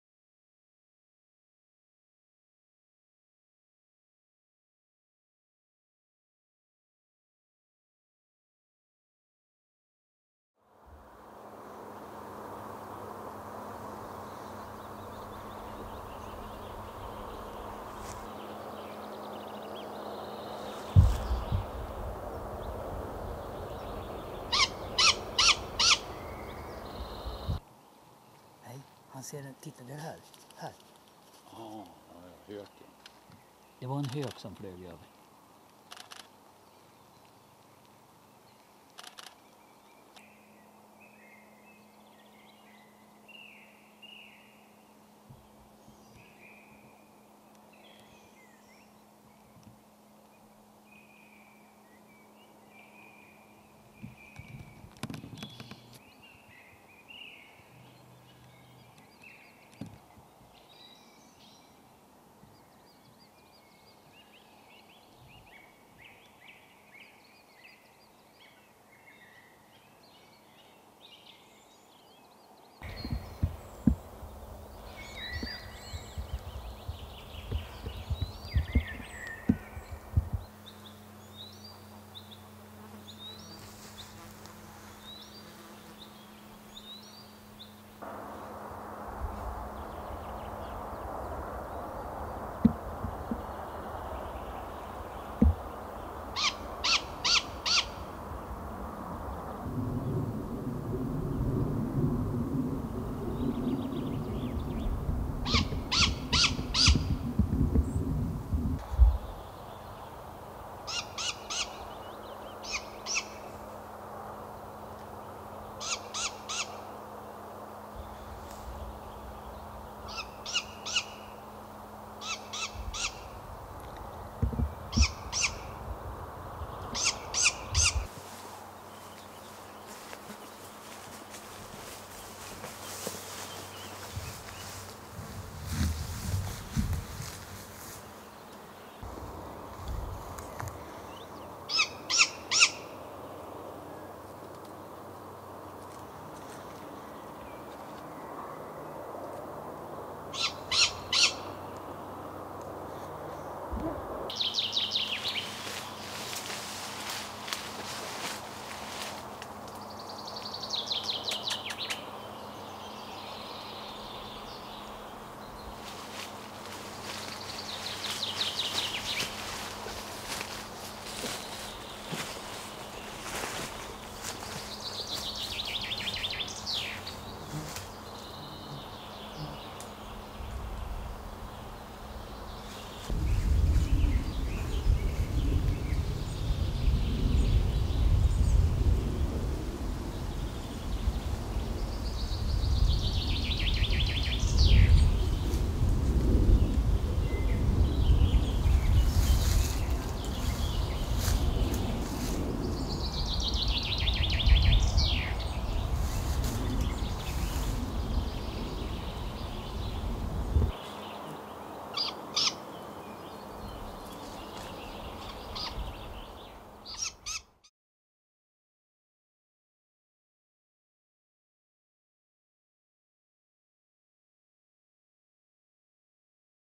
LAPPUGGLA Great Grey Owl (Strix nebulosa) Klipp 2203
Lappuggla. Juni 2010 i Västmanland. Great Grey Owl in Sweden,  AROS FILM Fotograf: Gunnar Fernqvist. Västerås. Sweden.   (122-Rf-238.)